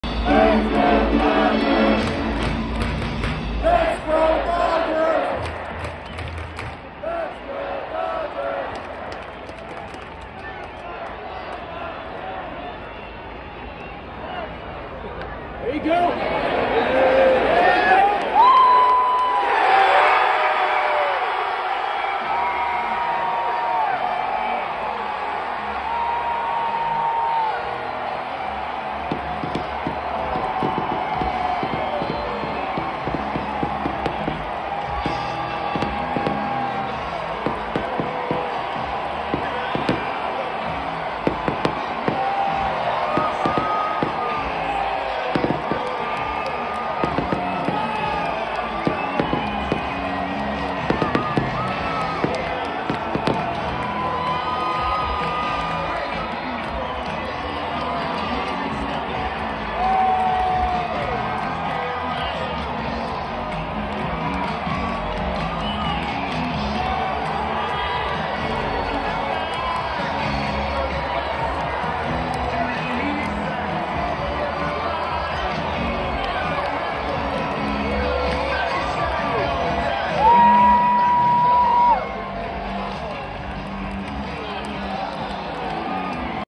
Mets @ Dodgers (10/20/24) 2024 NLCS Game 6: Final Out, Dodgers Win NL Pennant
The final out of the Dodgers clinching the National League pennant featuring fireworks.